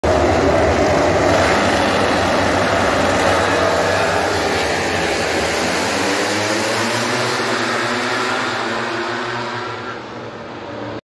Start MotoGP Race Sachsenring 2022 Sound Yamaha Honda Ducati Suzuki KTM
Start of the MotoGP Class Sachsenring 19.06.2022 from the Top of Pitlane.